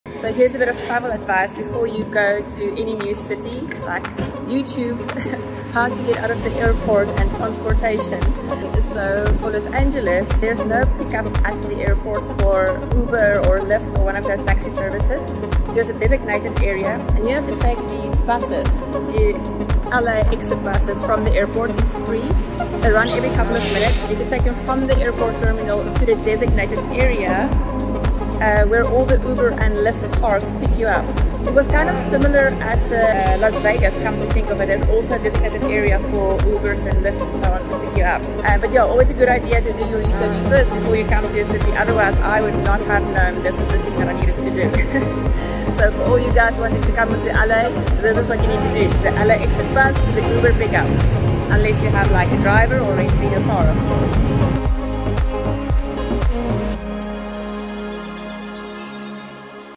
Can you get an Uber from LAX? | Los Angeles Airport | How to use the LAXit Bus Service
🌟 Navigating LAX: Ride the LAXit Bus to Uber/Lyft! 🚌✈️

When visiting the City of Angels, your journey may begin at the iconic Los Angeles International Airport (LAX)! 🌴✈️ If you're wondering how to smoothly transition from your flight to your Uber or Lyft ride, I've got you covered! 🚗💨

🚌 Step 1: Find the LAXit Bus Stop
After landing, follow the signs to the LAXit bus stop. It's conveniently located near each terminal, so you'll easily spot the designated area no matter where you arrive. 🚏

🚍 Step 2: Hop-On the LAXit Bus
Once you're at the bus stop, look out for the bright green LAXit buses. These shuttles transport passengers to the designated Uber and Lyft pickup zones. 🚌👍

🎫 Step 3: Board the Bus (It's Free!)
The best part? Riding the LAXit bus is absolutely FREE! Hop on board, and you'll go to your rideshare quickly. It's a quick and hassle-free way to get to your destination. 🆓🚌

📍 Step 4: Arrive at the Uber/Lyft Pickup Zone
Within a few minutes, the LAXit bus will drop you off at the designated Uber/Lyft pickup zone. You'll find it well-organized and easy to navigate, making it a breeze to meet your ride. 🚗🚙

✅ Bonus Tip: Check Your App
While you're on the bus, take a moment to open your Uber or Lyft app. Confirm your pickup location and make any necessary adjustments to ensure a smooth and timely pick-up.

🤳 Step 5: Enjoy Your Ride!
With your driver en route, all left to do is relax and anticipate your LA adventure. The journey starts here whether you're headed to the beach, Hollywood, or downtown! 🌟🌆

So there you have it! Riding the LAXit bus is the hassle-free way to transition from the airport to your Uber or Lyft. Snap a selfie on the bus, share your excitement, and let the world know you're ready to explore the City of Angels! 📸🌇

📚 Content
0:00 - Ride the LAXit Bus to Uber/Lyft!